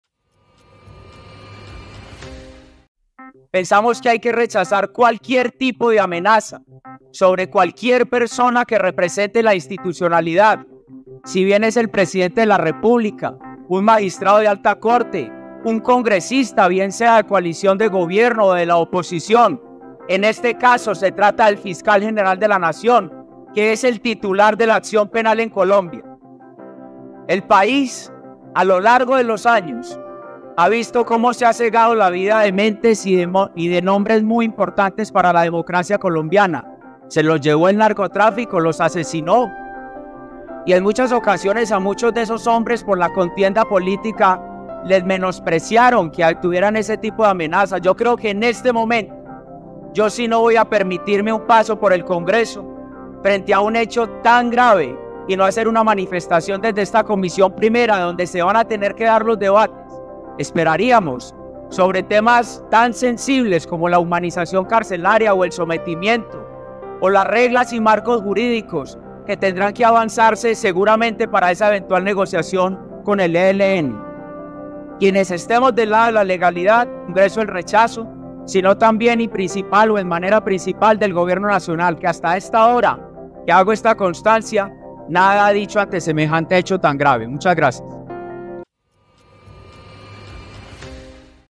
UN HECHO TAN GRAVE 🔴 FUERTE LLAMADO DE ATENCIÓN HERNÁN CADAVID AL GOBIERNO
Desde la comisión I de la Cámara, Hernán Cadavid